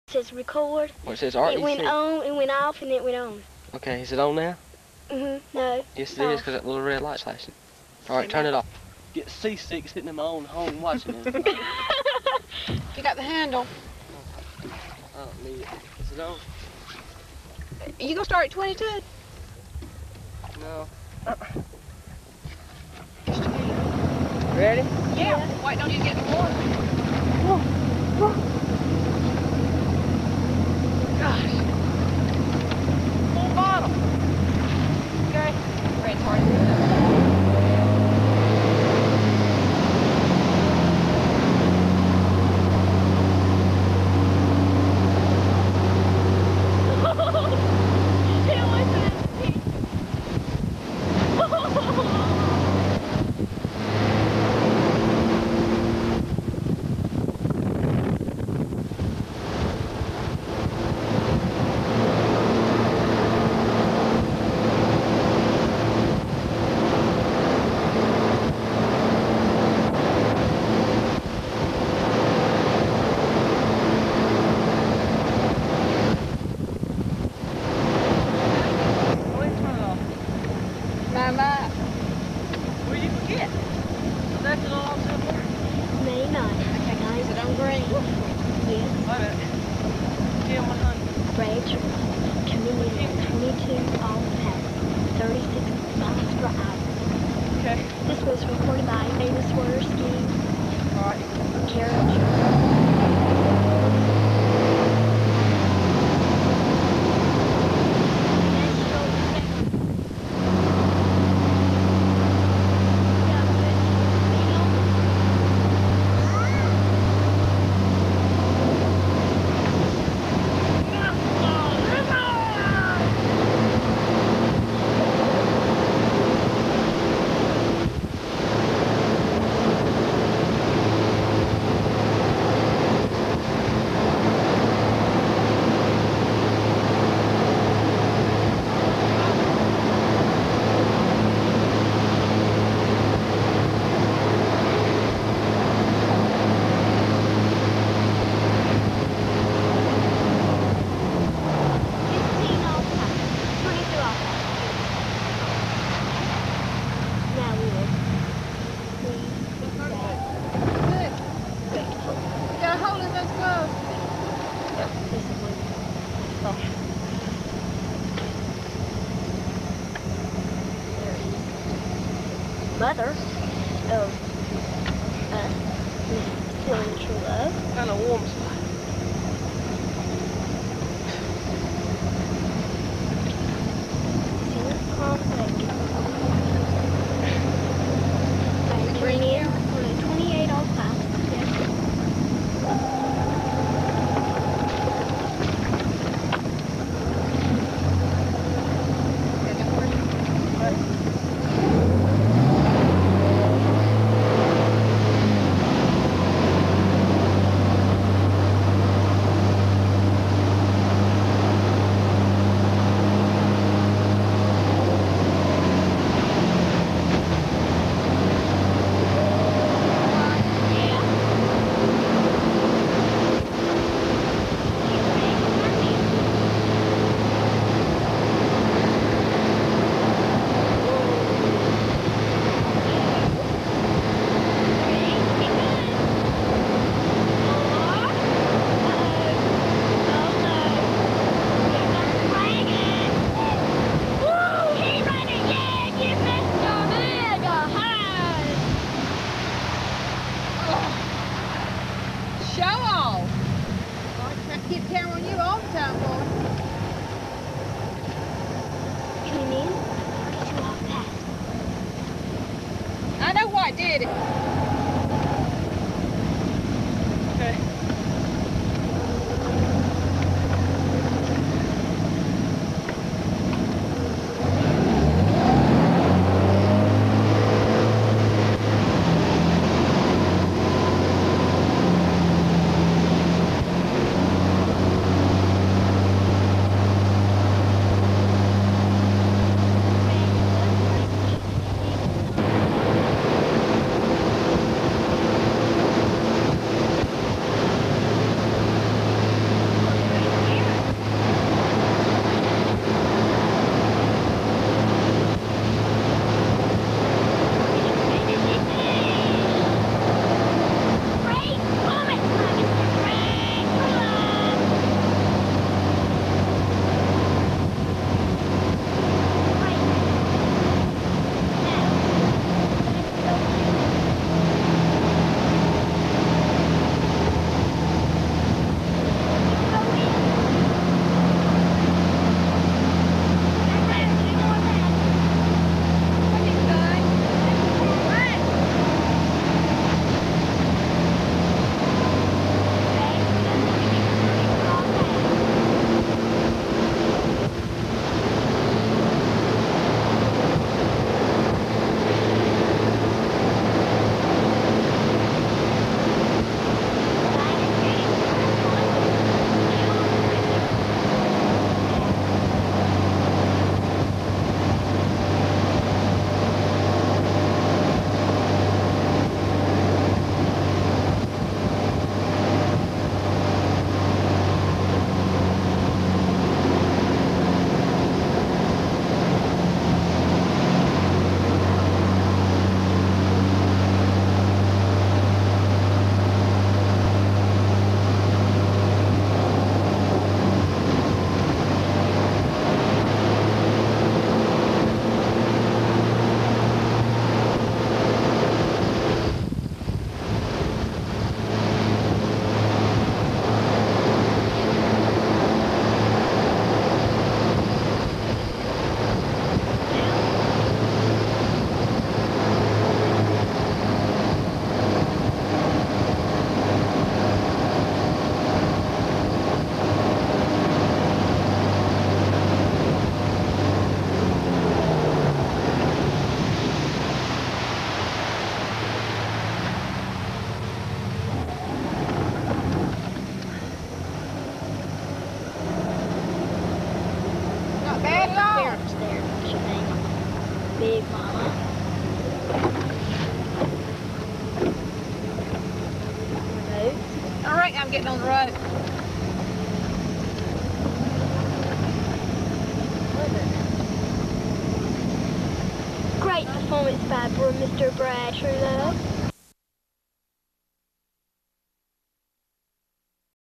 1984 Slalom Run
Old 1984 video footage of a few early season slalom passes from Youngs Pond in Angier, NC.